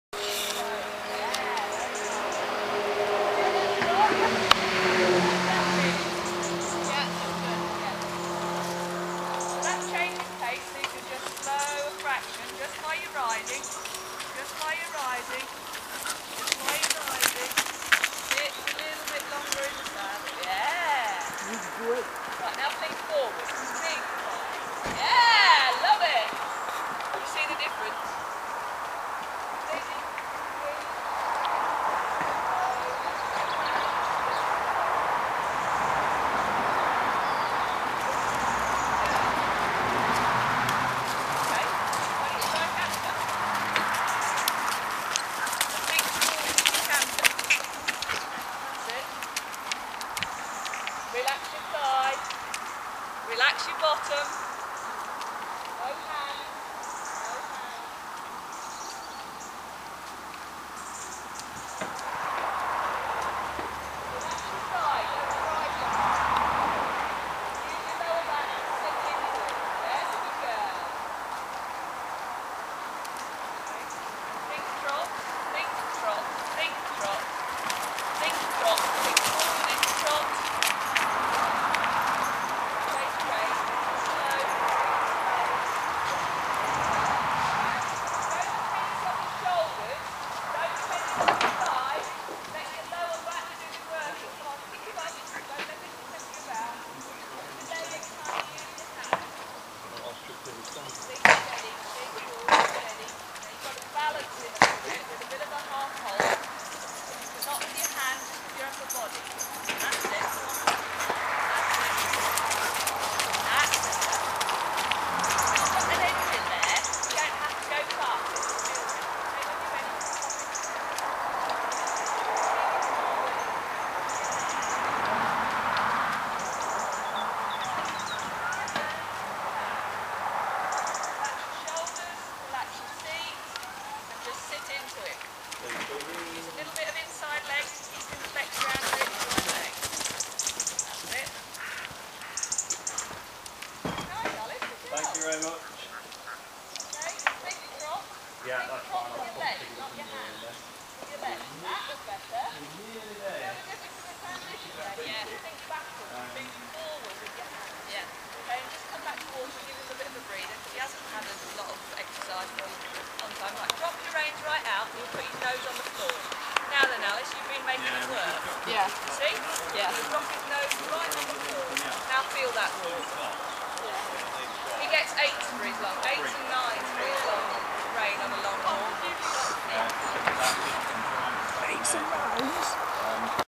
Highpoint Maverick
16.2hh, British warmblood gelding and we're getting him :) but Hob is being re homed :'(